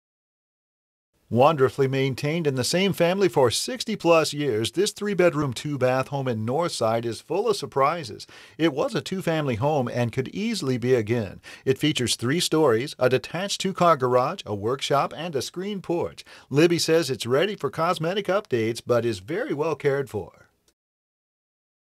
Home for sale in Northside, OH | $50,000
Wonderfully maintained home in same family for 60+ years- full of surprises! Was a 2 family, easily could be again. 3 stories, det 2-car garage, workshop &amp; screened in porch. Ready for cosmetic updates, but very well cared for.